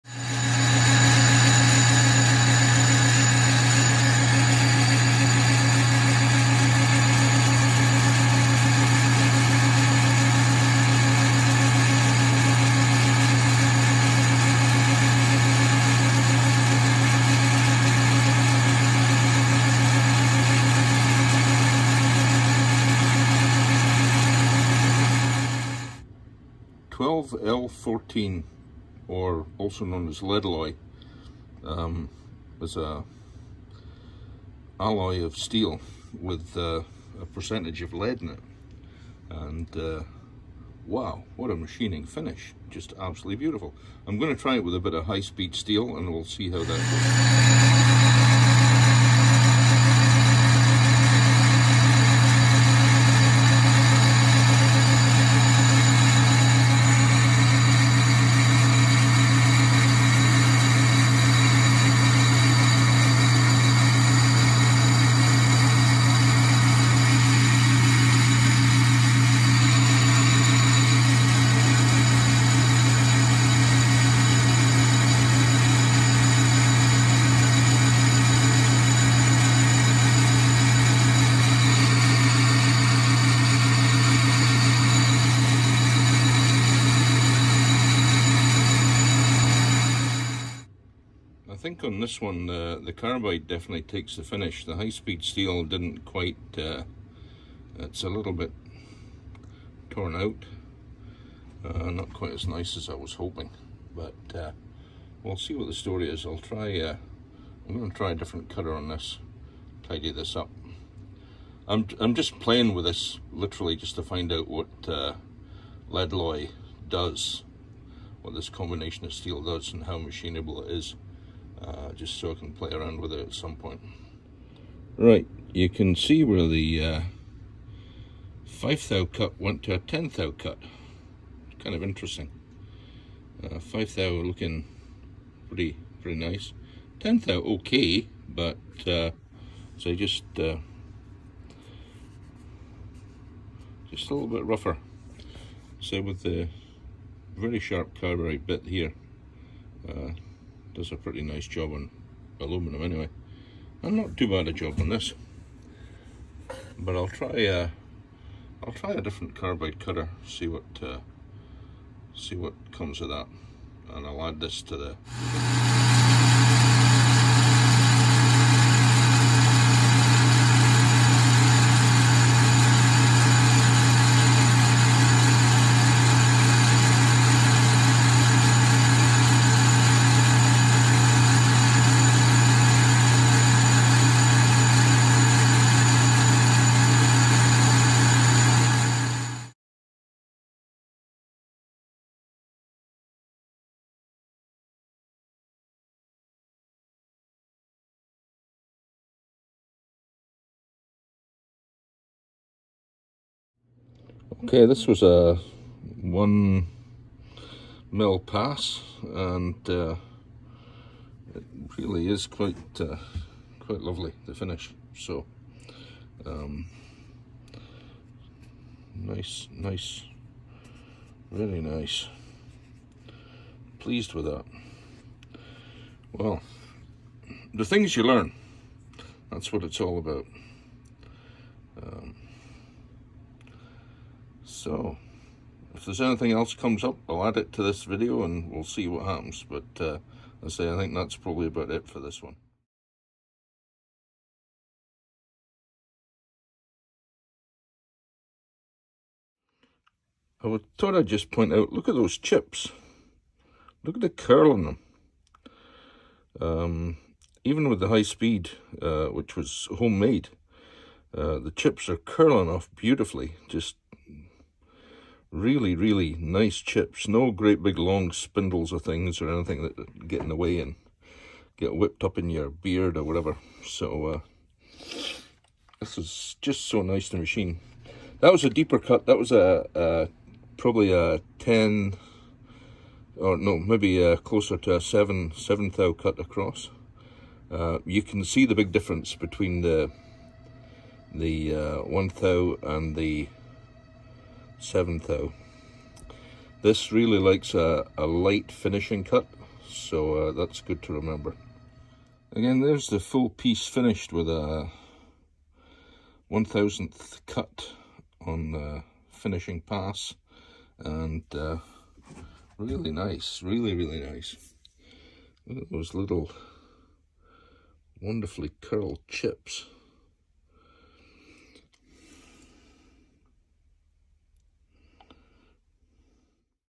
Machining Leadloy or 12L14 steel.
Few different samples of machining 12L14 steel.